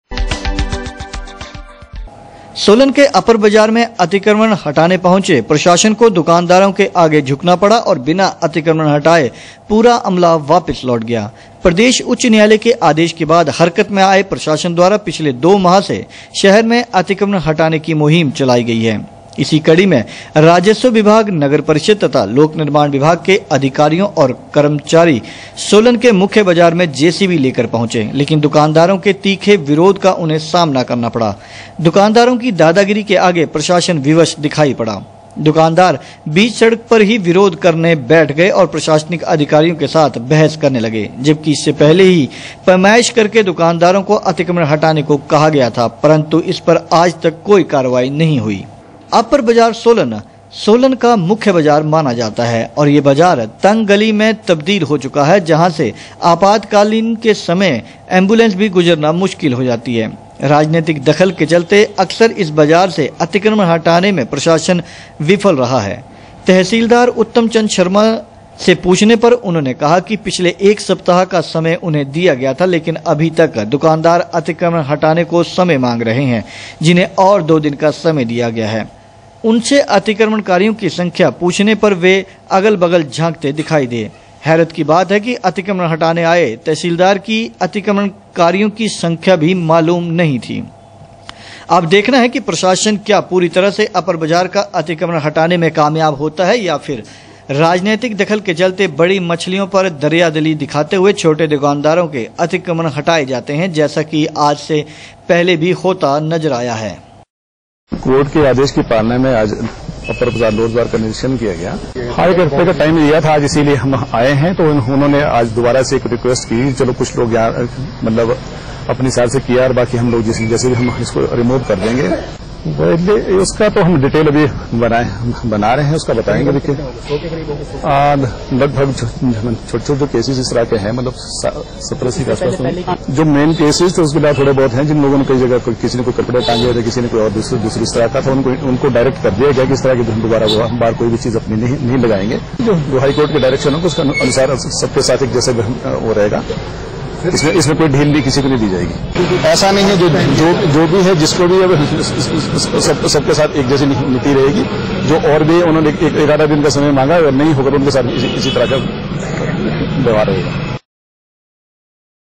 प्रदेश उच्च न्यायालय के आदेश के बाद हरकत में आये प्रशासन
प्रदेश उच्च न्यायालय के आदेश के बाद हरकत में आये प्रशासन द्वारा पिछले दो माह से शहर में अतिक्रमण हटाने की मुहिम चलाई है। इसी कड़ी में आज राजस्व विभाग, नगर परिषद तथा लोक निर्माण विभाग के अधिकारी और कर्मचारी सोलन के मुख्य बाजार में जे सी बी लेकर पहुंचे। लेकिन दुकानदारों के तीखे विरोध का उन्हें सामना करना पड़ा।